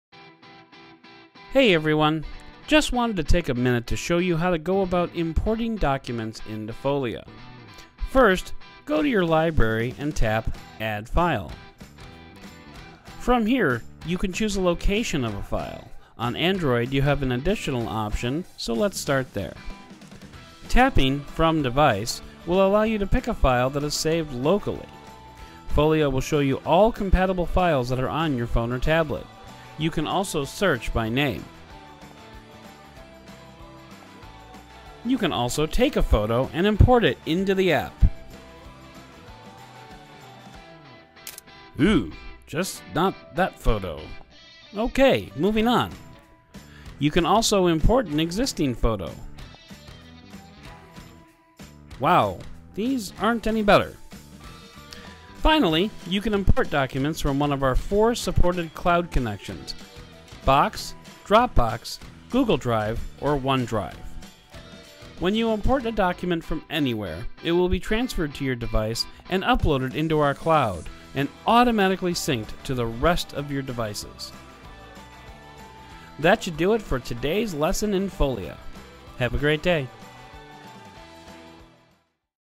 How to Import Documents into Folia
A short video about how to import documents into Folia, the next generation annotation app for iOS and Android.